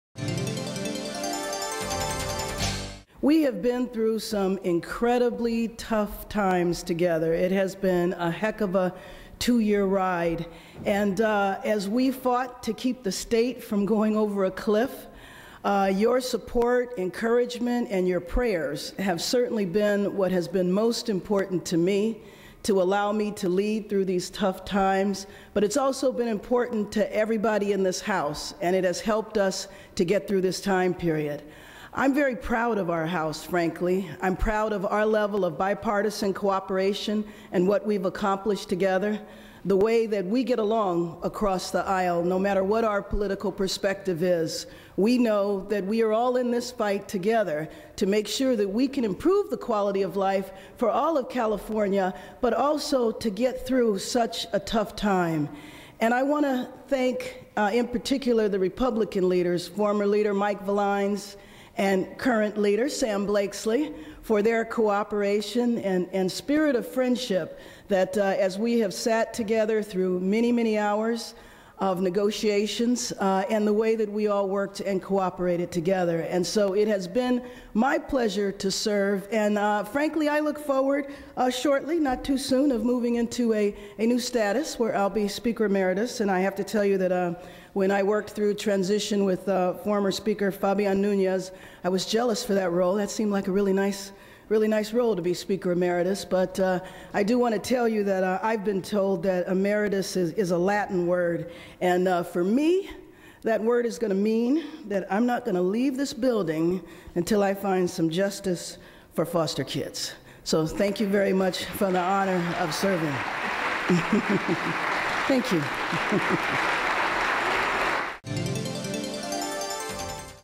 Speaker Bass Preparing to Give Gavel to Speaker-elect Pérez
(Sacramento)  The members of the California State Assembly have elected John A. Pérez (D-Los Angeles) to be the next Speaker of the Assembly. Current Speaker Karen Bass (D-Los Angeles) took a few moments to comment on her tenure as Speaker just prior to the Assemblys vote. Here are her comments in this Assembly Access video. The timing of the transition from current Speaker Bass to Speaker-elect Pérez has not yet been finalized.